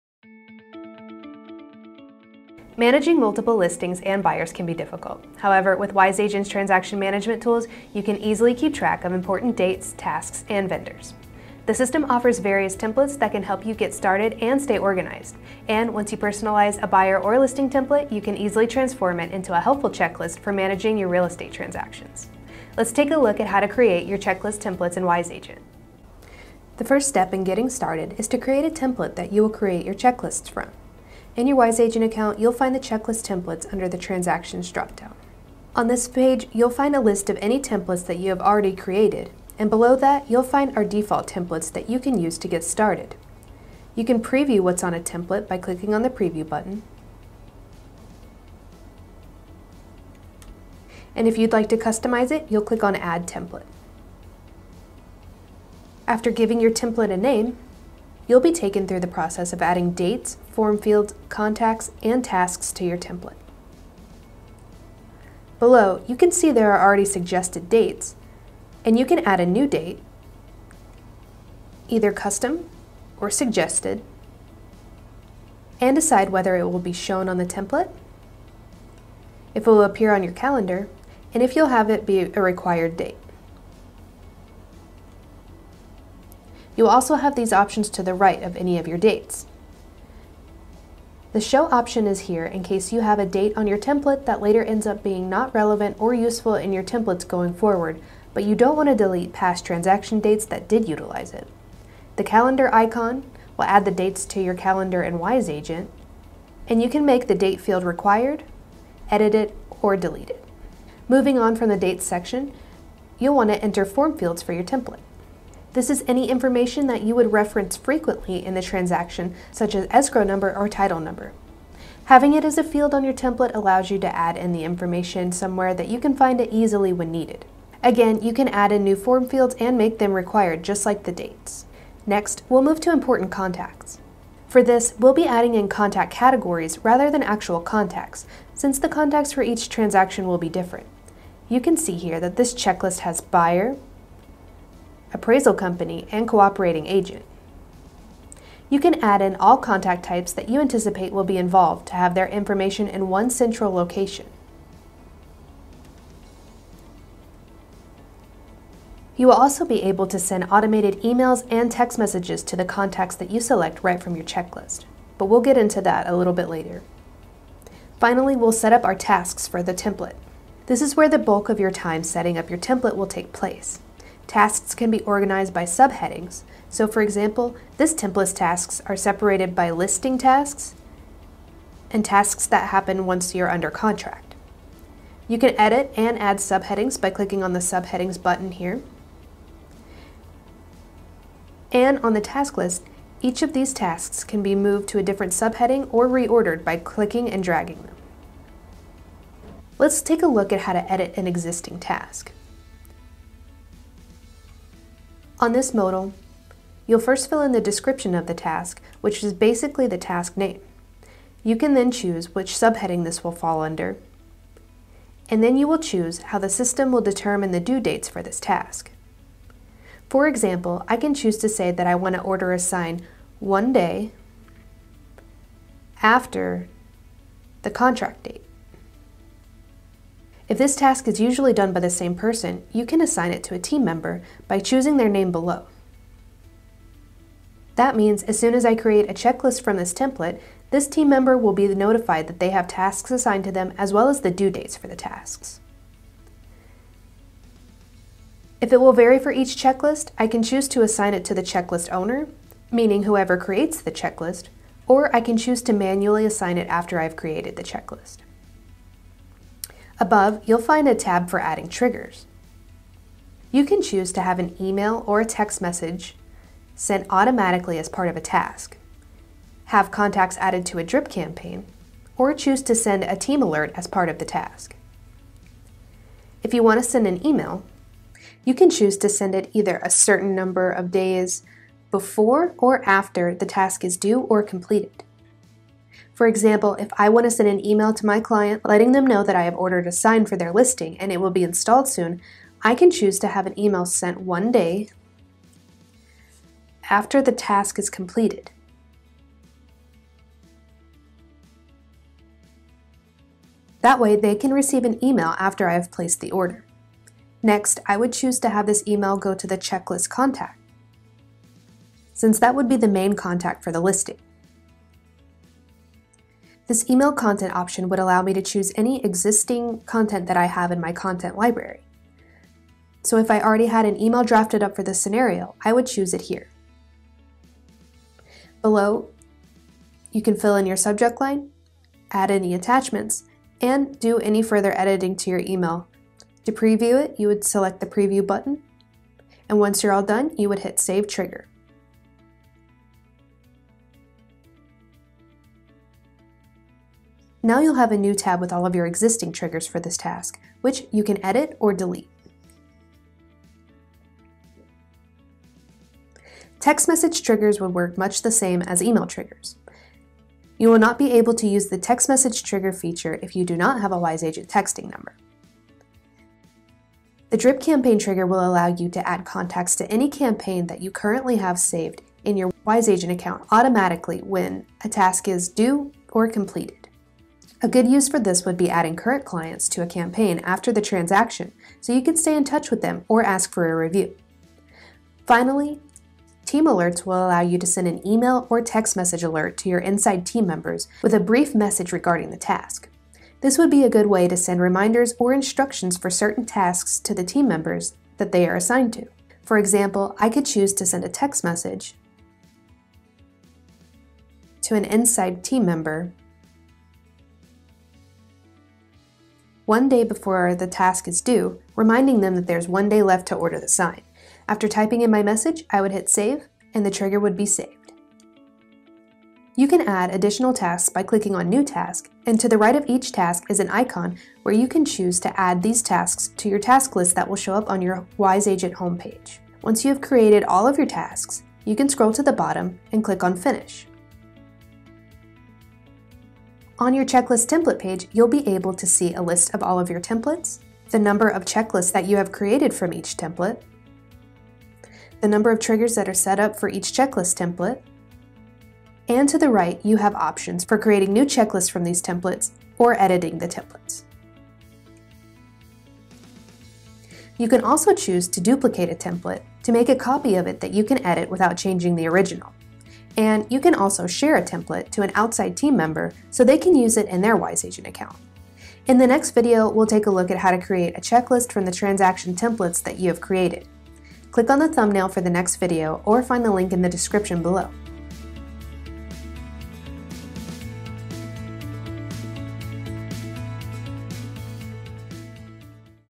Creating Templates for Checklists in Wise Agent
Managing multiple listings and buyers can be challenging. However, Wise Agent's real estate transaction management tools make it easy to keep track of important dates, tasks, and vendors.

The system offers various templates to help you get started and stay organized. Once you personalize a buyer or listing template, you can quickly transform it into a helpful checklist for managing your real estate transactions.

Follow along with us as we guide you through setting up a template for your checklists and how to include automated email and text messages throughout the process.